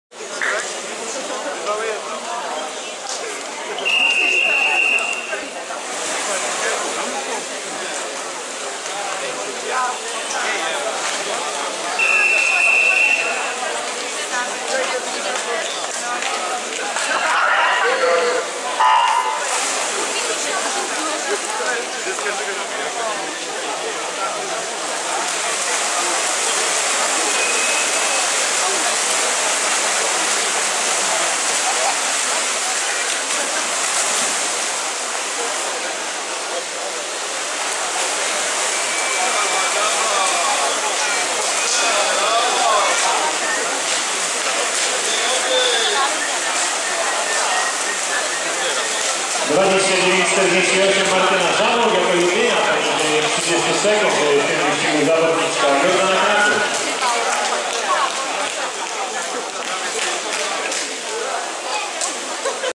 P. Koscik 50 grzbietowy Grand Prix Puchar Polski Krakow 25-26.10.2014
1 Reks Aleksandra 00 G-8 Bielany Warszawa 30.83
2 Górecka Weronika 00 Juvenia Wrocław 30.40
3 Wolny Magdalena 98 AZS AWF Katowice 29.96
4 Żarłok Martyna SK 97 Jordan Kraków 29.38
5 Piechocka Jagoda 90 AZS AGH Kraków 29.57
6 Bernat Gabriela 99 Skarpa Lublin 30.31
7 Koluch Julia 00 Kapry-Armexim Pruszków 30.74
8 Kościk Patrycja 98 Sparta Biłgoraj 30.85